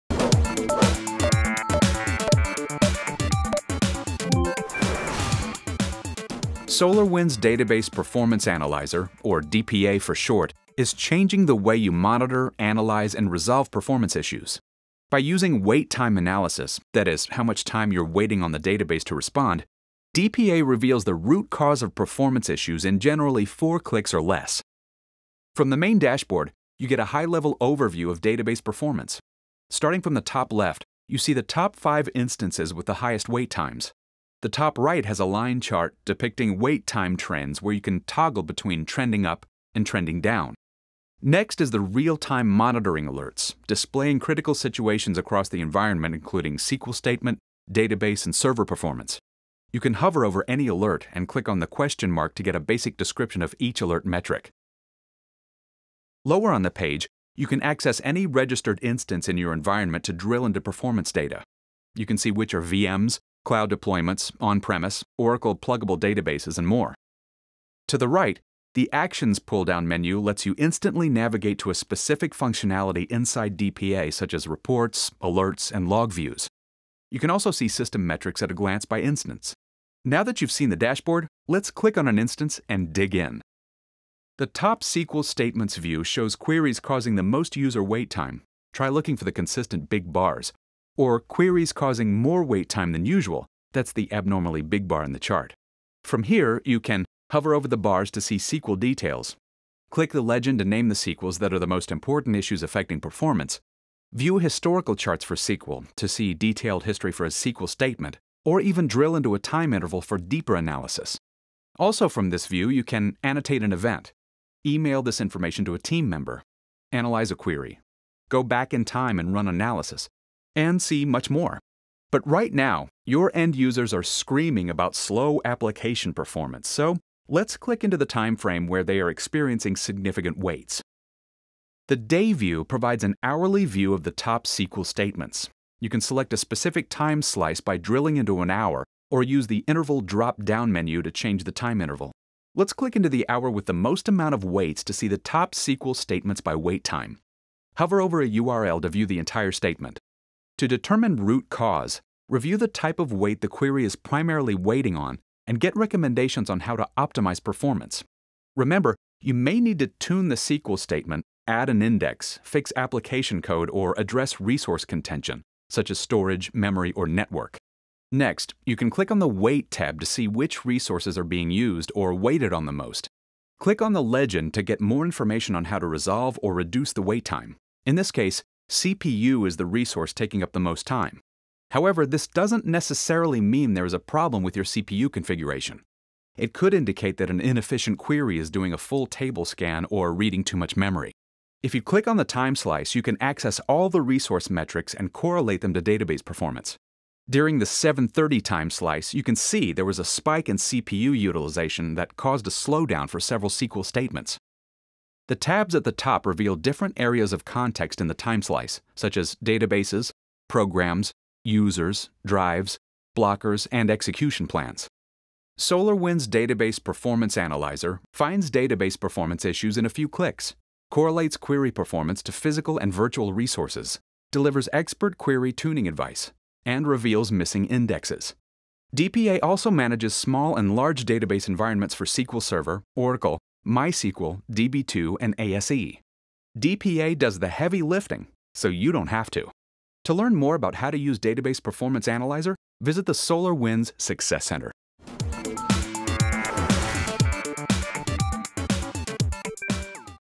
How to Navigate the DPA UI to Diagnose Database Performance Issues
This video demonstrates how to quickly diagnose database performance issues using wait time analysis with Database Performance Analyzer.